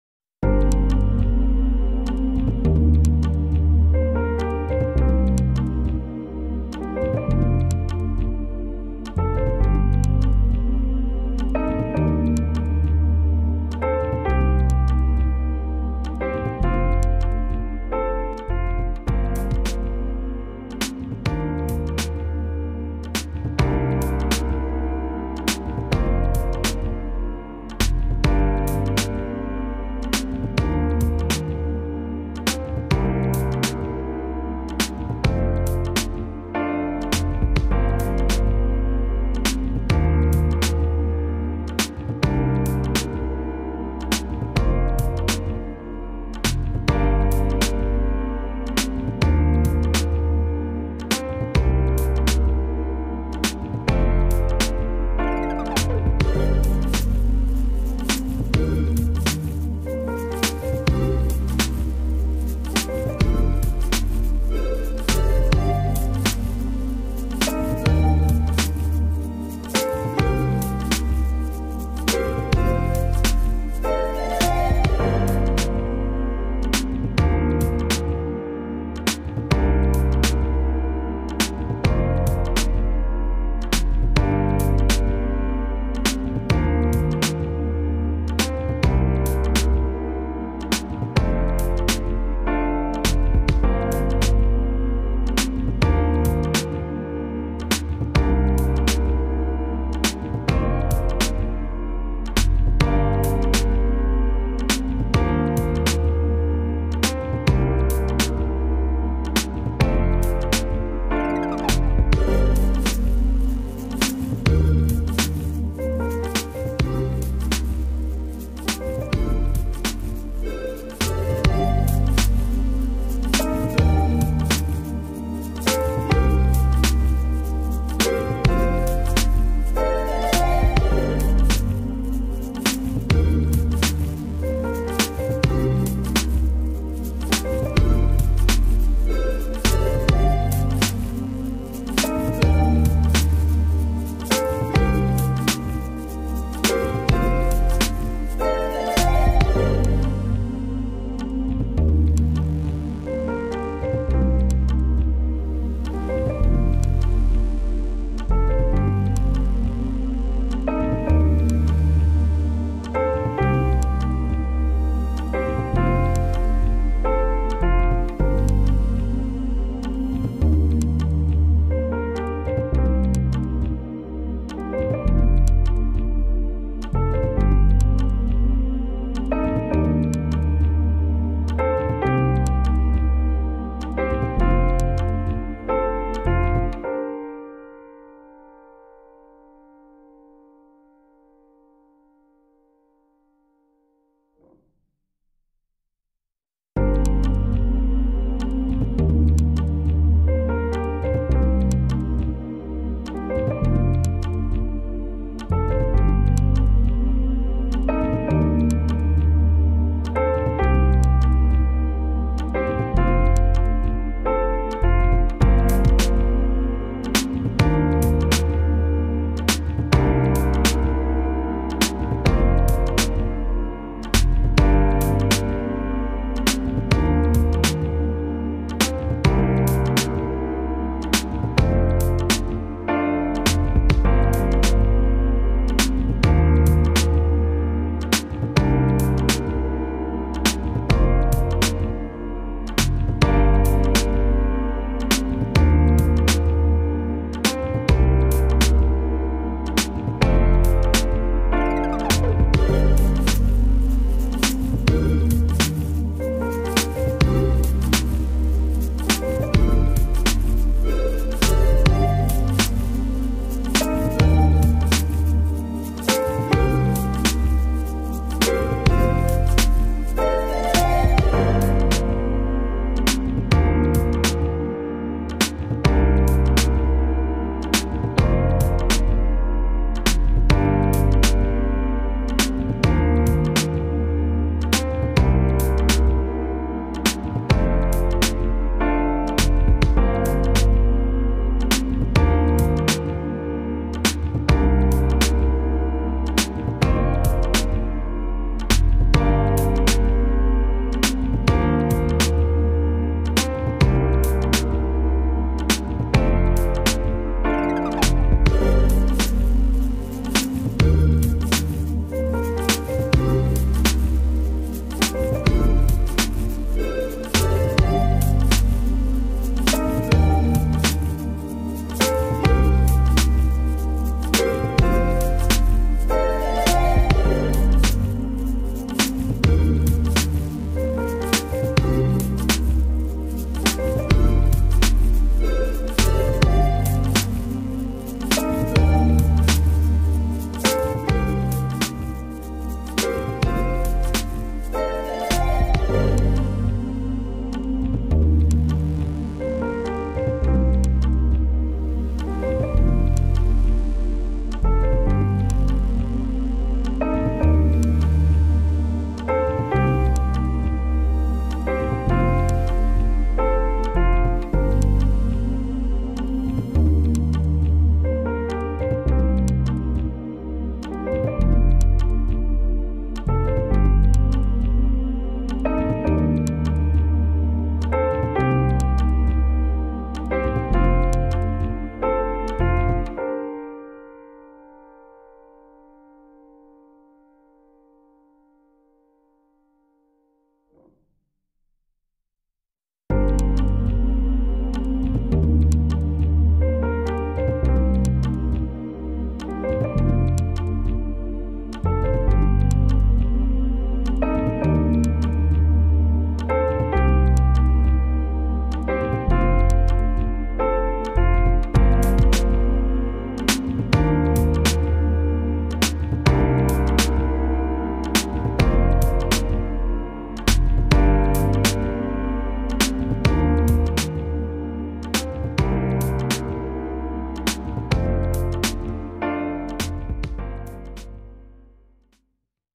How to draw scenery of alone man with soft pastel step by step for beginners.
I used camel soft pastel in this drawing.

Alone man drawing with soft pastels

Buy soft pastel :-